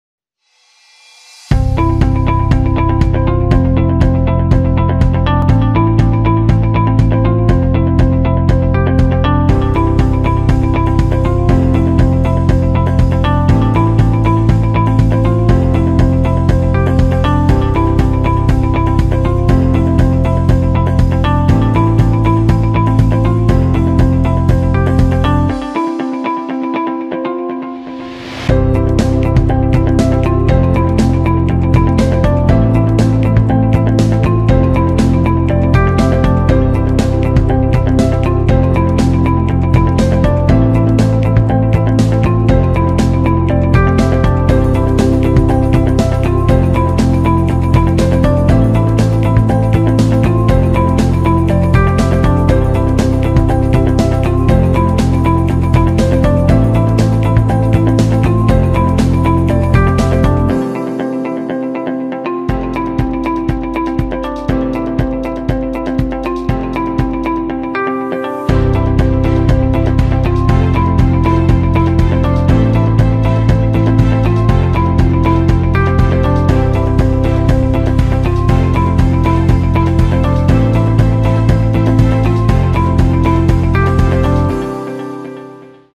Experience the Apartment as One's Own Home | Layer View @ Own A Space
Experience the Layer View at Own A Space’ online real estate portal! A Layer View is a great way for property buyers to take a virtual look through the houses and projects, see every minute details of the floor plan and enjoy a smooth and consistent buying experience.